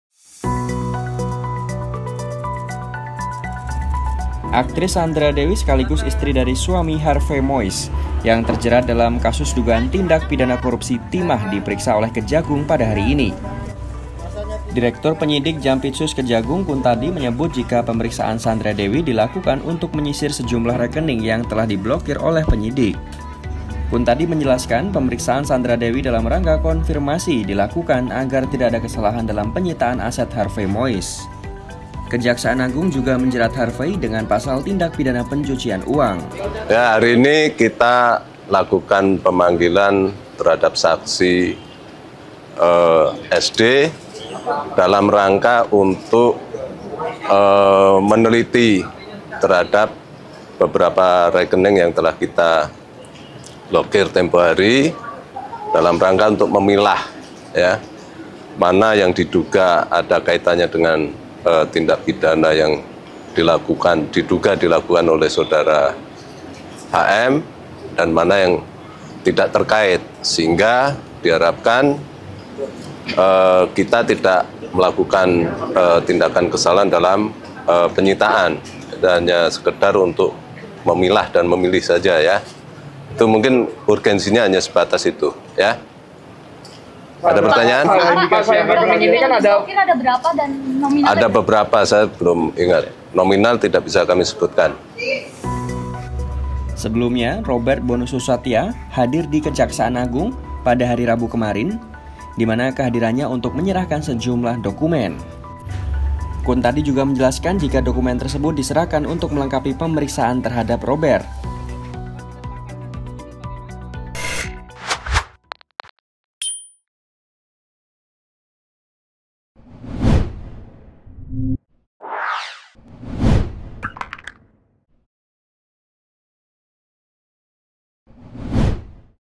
TOP NEWS OF THE DAY : Periksa Sandra Dewi, Kejagung Sisir Beberapa Rekening yang Diblokir
Kamis (04/04/2024) Jakarta, IDN Times - Kejaksaan Agung (Kejagung) memeriksa aktris Sandra Dewi dalam perkara dugaan tindak pidana korupsi timah yang menjerat sang suami, Harvey Moeis, Kamis (4/4/2024).

Baca selengkapnya di sini:
?utm_source=youtube&utm_medium=youtube&utm_campaign=youtube